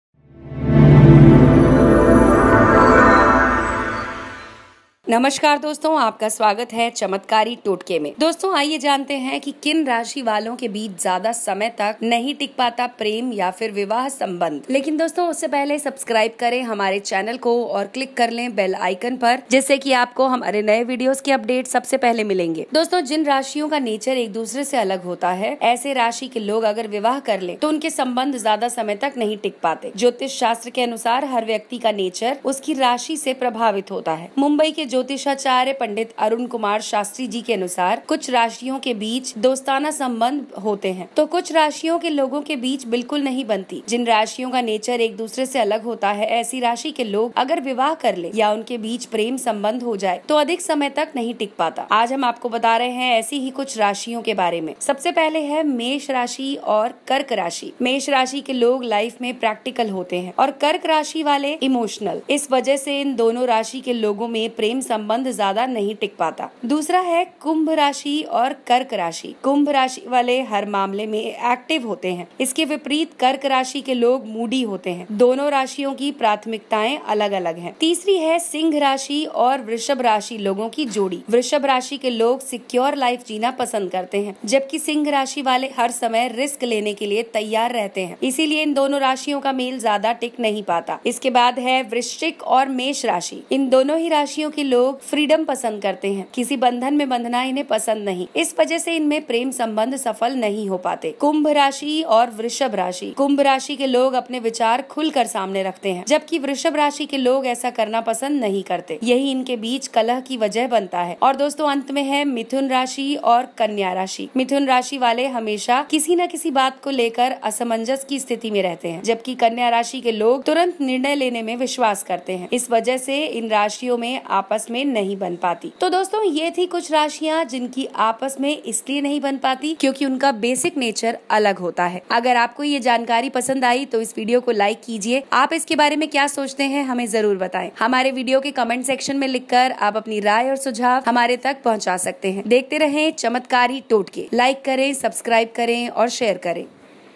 किन राशि वालों के बीच ज्यादा समय तक नहीं टिक पाता प्रेम और विवाह संबंध | According to Astrology
किन राशि वालों के बीच ज्यादा समय तक नहीं टिक पाता प्रेम या विवाह संबंध